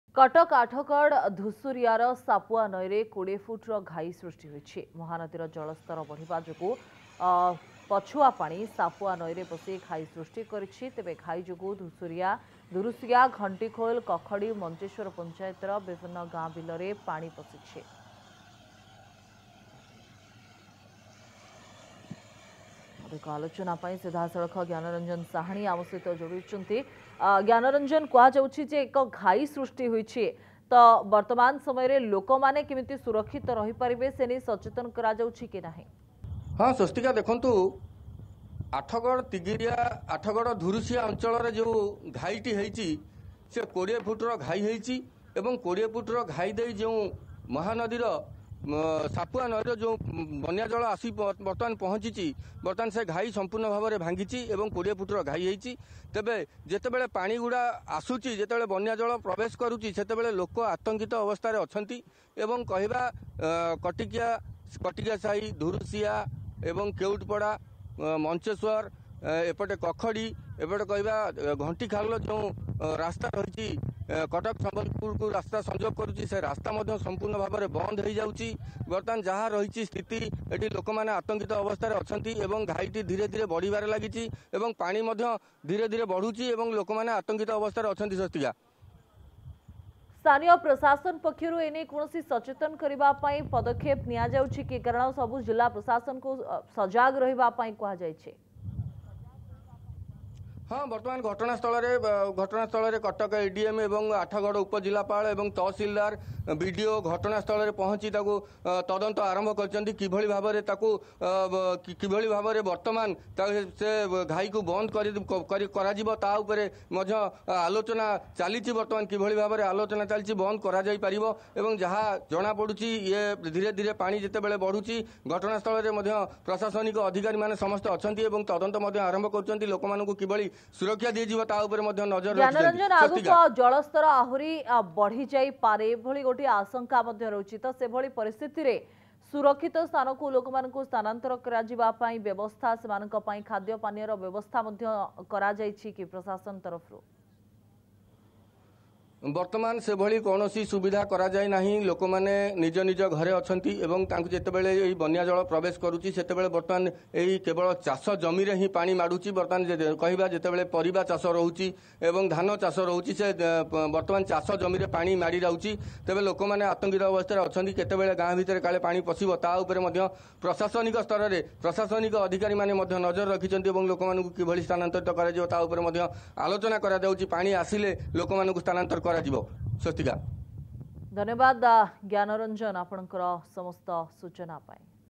20 ft Breach Occurred In Sapua River In Athagarh
20 ft Breach Occurred In Sapua River In Athagarh